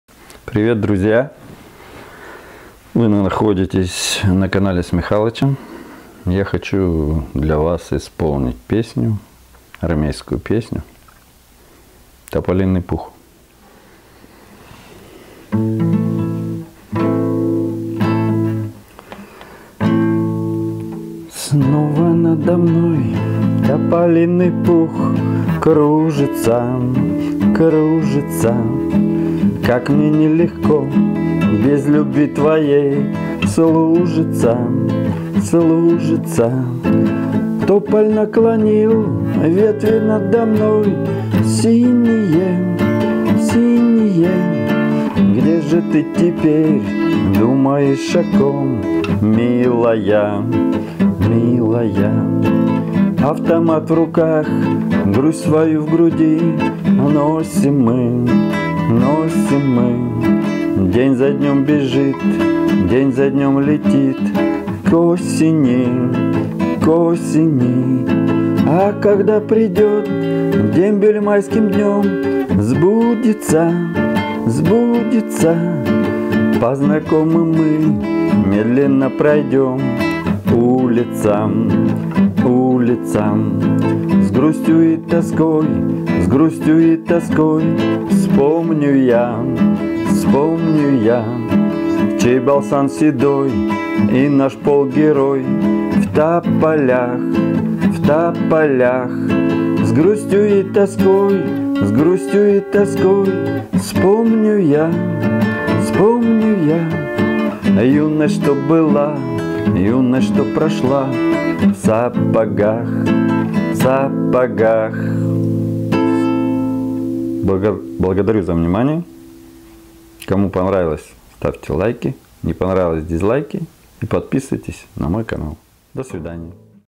Армейская песня
Добрый день друзья. Вспомнил армейские годы в день Защитника Отечества и решил спеть эту песню. Службу в рядах С.А. на территории дружественной Монголии. Армейские будни.